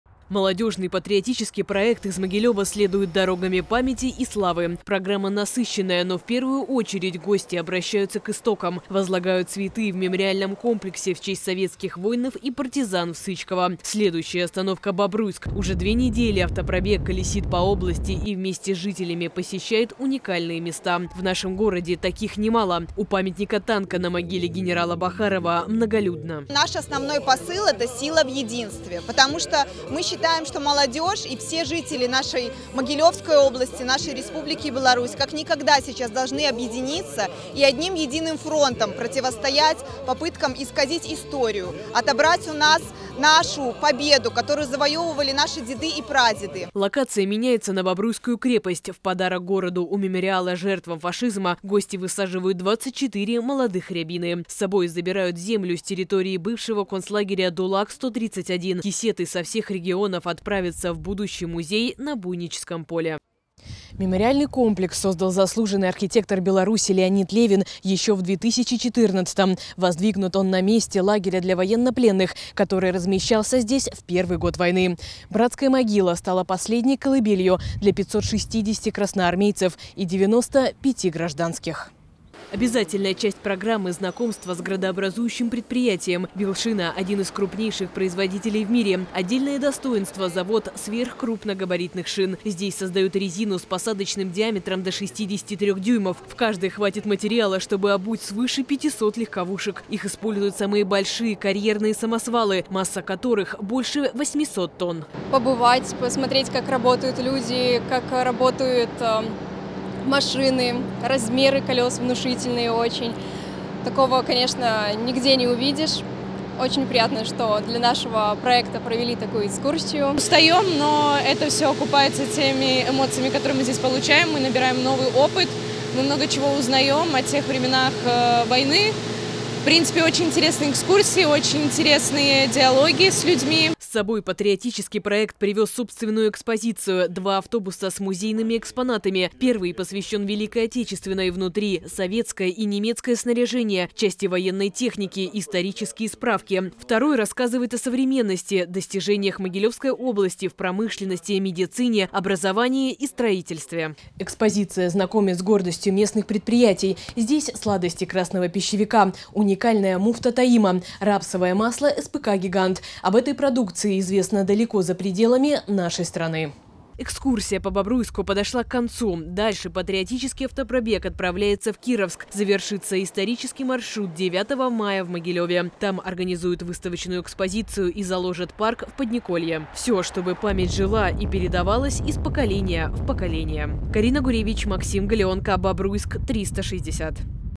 Исторический маршрут
Бобруйск встретил участников молодежного патриотического проекта. 30 ребят посетили места боевой славы, познакомились с культурной и промышленной жизнью города. С собой привезли собственную экспозицию – два специально оформленных автобуса.